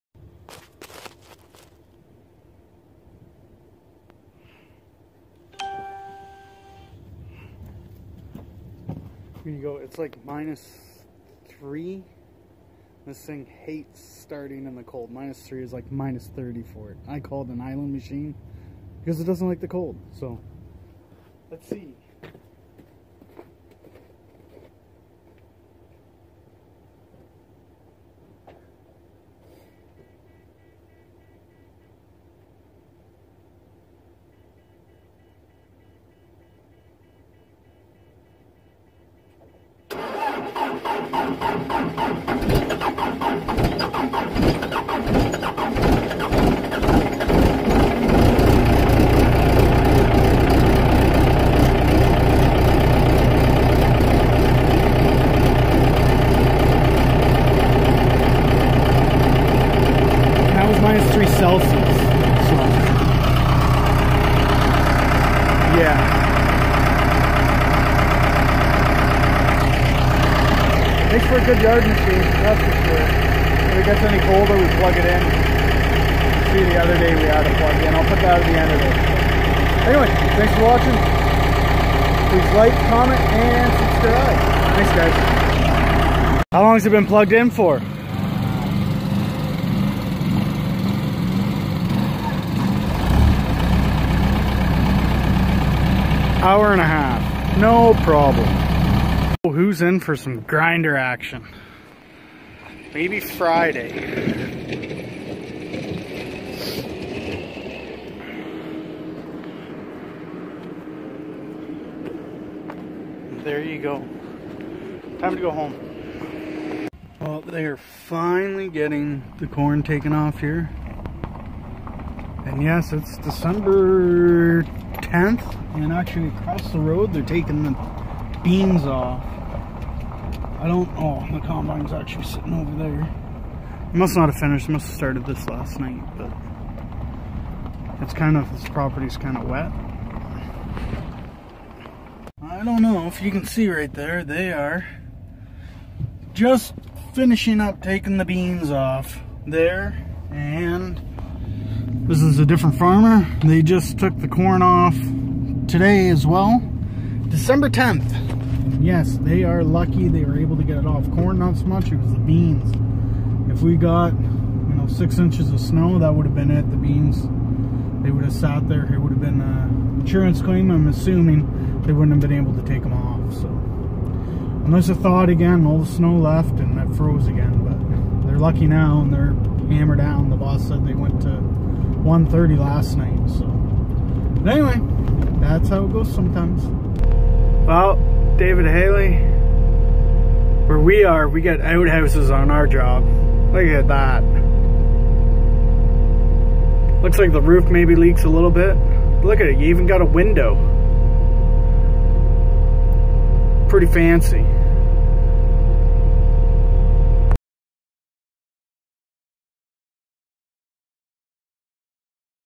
60XT COLD START and some randoms
Just showing you the case 60xt hates the cold and yes they are still taking crops off here this week of dec 10th.